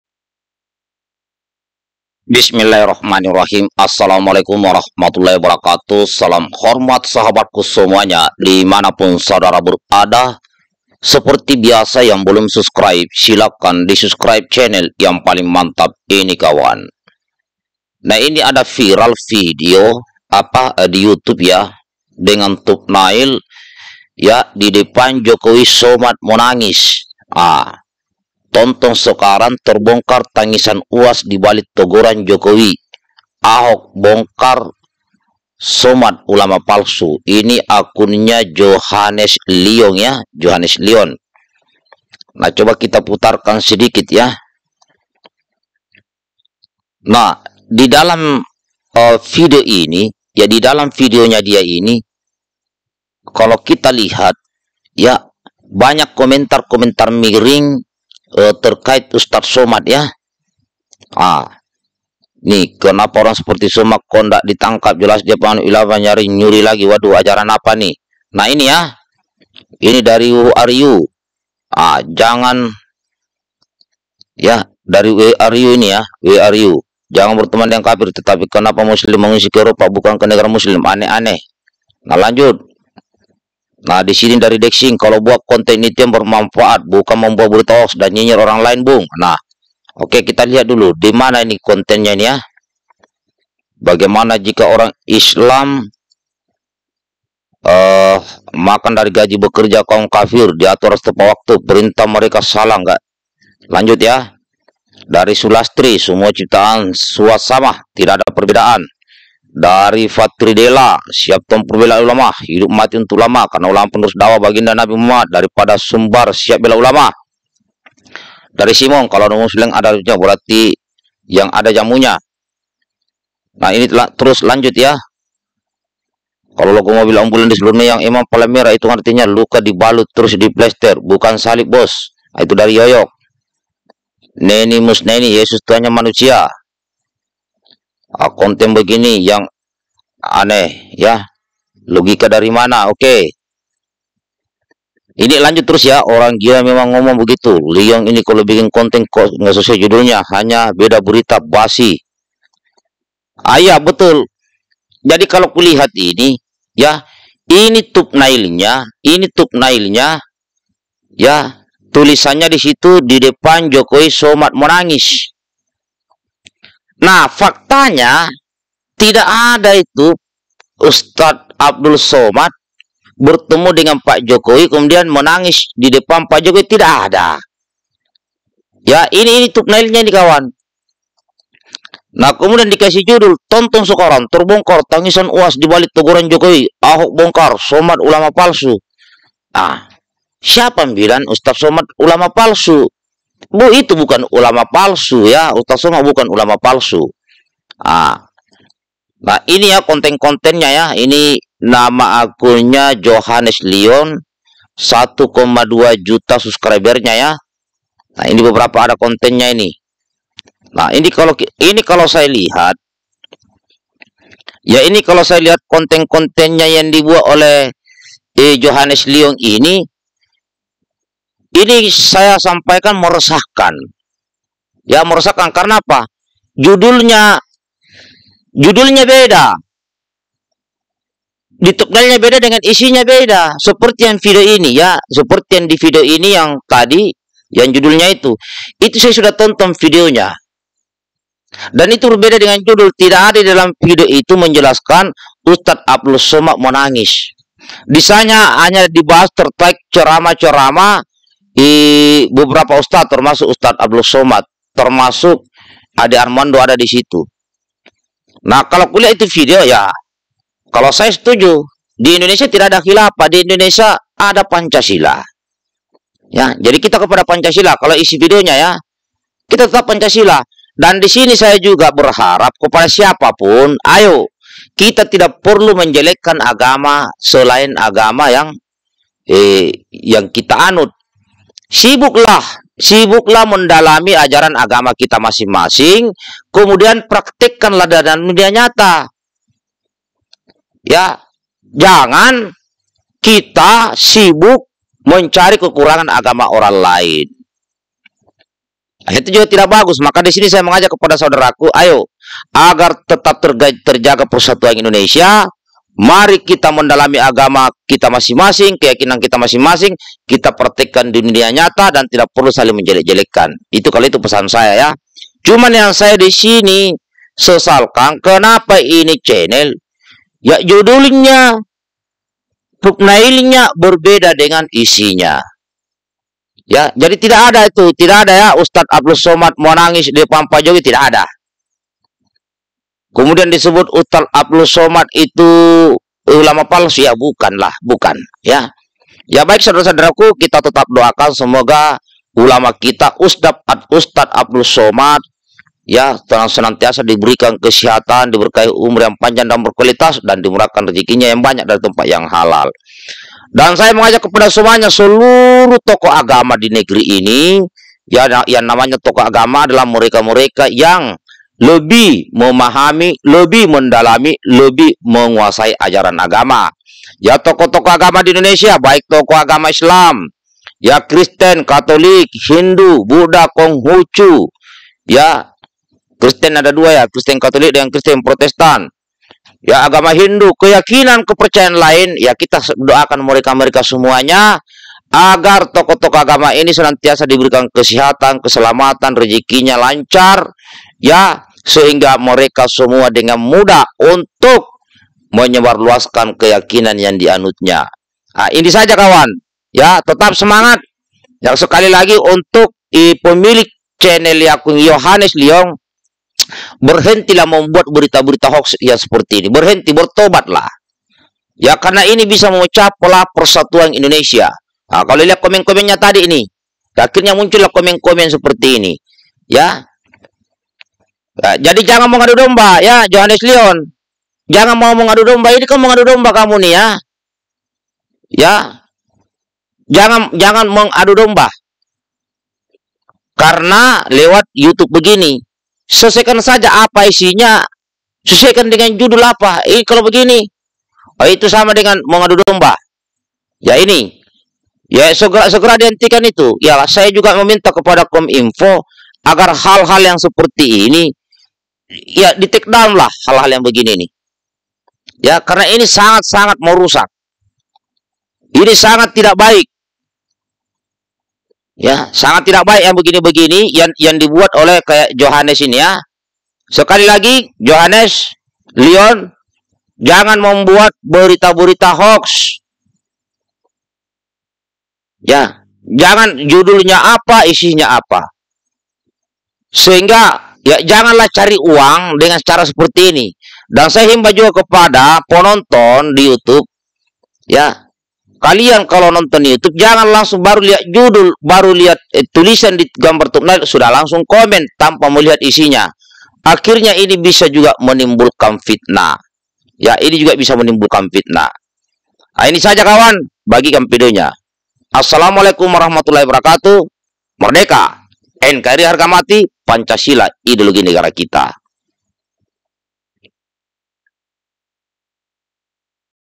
CEK FAKTA....VIRAL : TANGISAN UAS DIBALIK TEGURAN JOKOWI, AHOK BONGKAR ULAMA PALSU.MPS Angkat Bicara
CEK FAKTA.
VIRAL : 🔴TONTON SEKARANG!!
TERBONGKAR TANGISAN UAS DIBALIK
TEGURAN JOKOWI,
AHOK BONGKAR SOMAD ULAMA PALSU.
MPS Angkat Bicara

Aamiin.

TTD